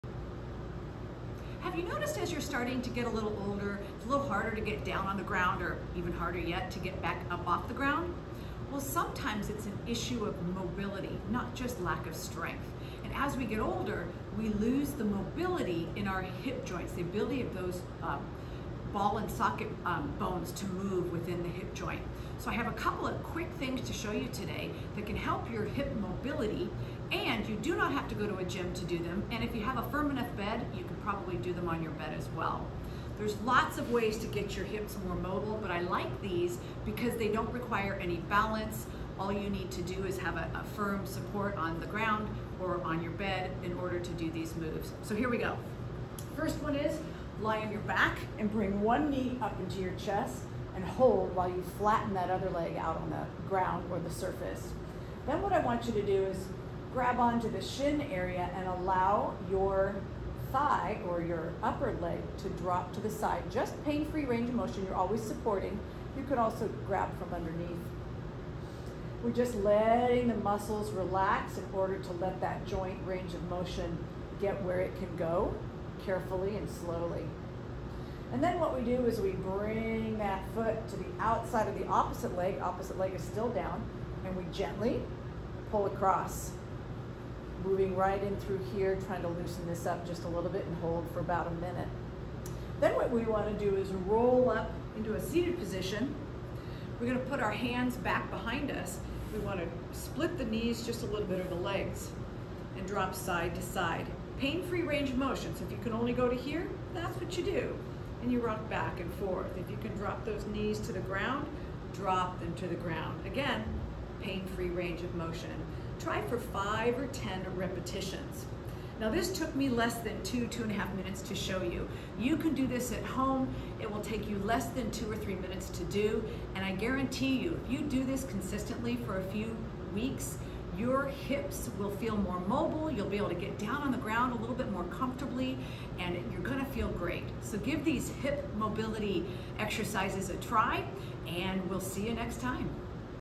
Simple ground-based hip mobility exercises
Do you feel stiff in  your hips?  In this video, Coach Becky shows you a few ground- based hip mobility exercises you can do in less than 3 minutes.
Practice these simple moves consistently for a few weeks and you'll find your hip joints feel less stiff!